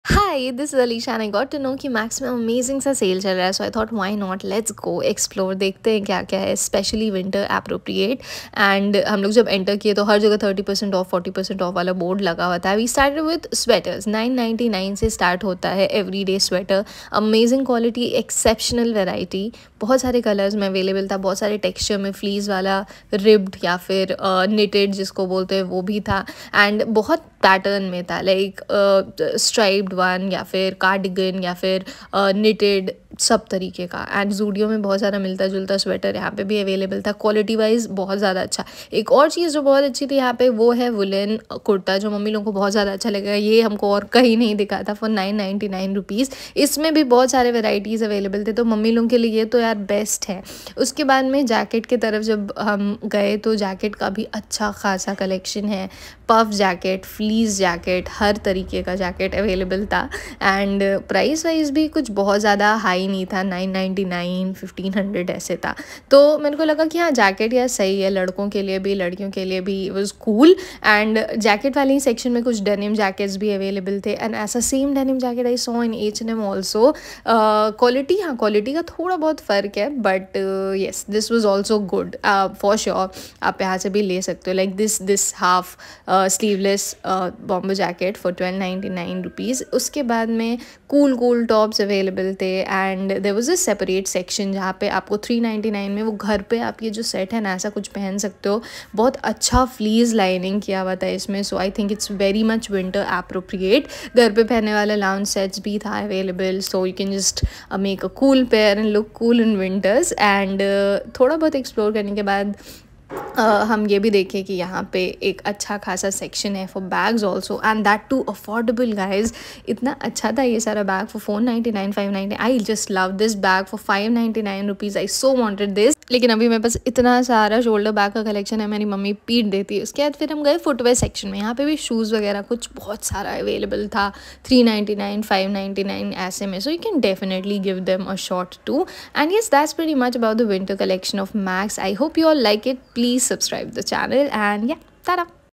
Max Vs. Zudio | Latest winter collection | Alisha singh
Recently i went to max to look into their winter collection. I was so surprised, firstly because of the sale and second of all, the winter collection.
Totally loved it, quality wise and amazing variety.
Hope you like this video. Also, i have recently posted vlogs of hm, zudio winter collection and hoping a good response on this one too.